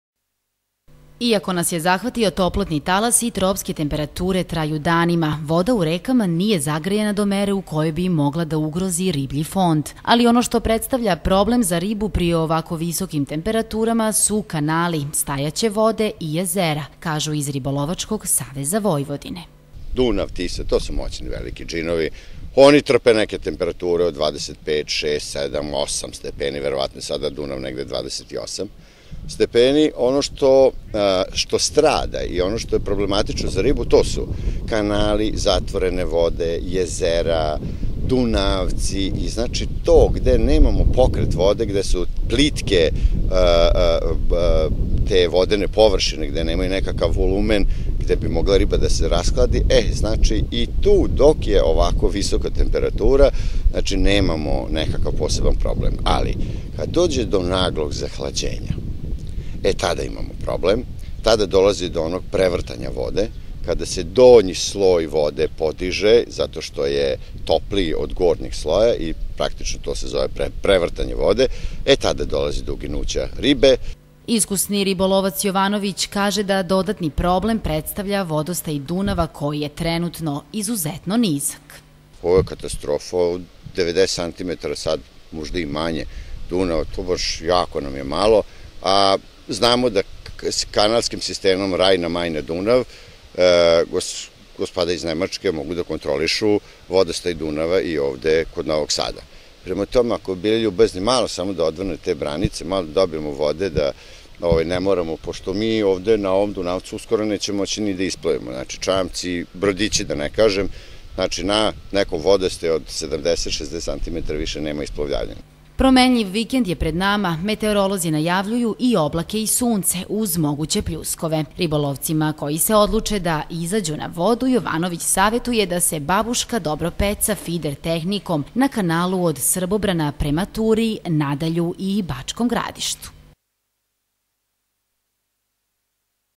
TV KANAL 9, NOVI SAD: VODOSTAJ DUNAVA IZUZETNO NIZAK
Ribolovci iz Ribolovačkog saveza Vojvodine ljubitelje pecanja, gledaoce Kanala 9, i ovog petka savetuju gde mogu da odu na pecanje tokom vikenda koji je pred nama.